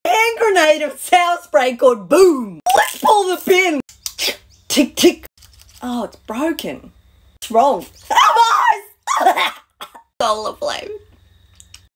Bomb Fail - RIP my eyes
Ok ok so this sour spray candy bottle in the shape of a grenade seemed fun